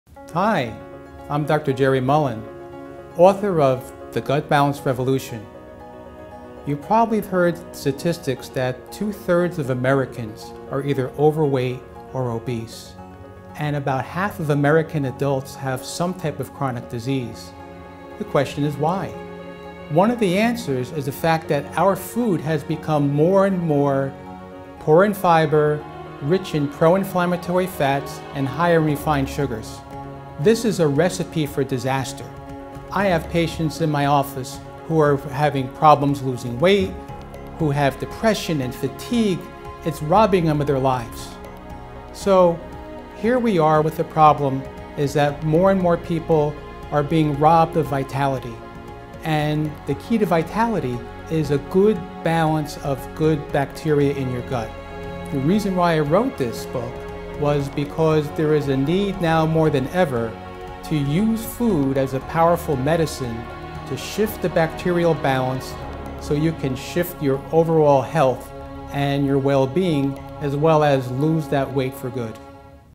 Your Gut Microbiome - the Key to Weight Loss
Shift the bacterial balance in your gut and you can create a fat burning microbiome. How? The right food is powerful medicine. I invite you to join the Gut Balance Revolution.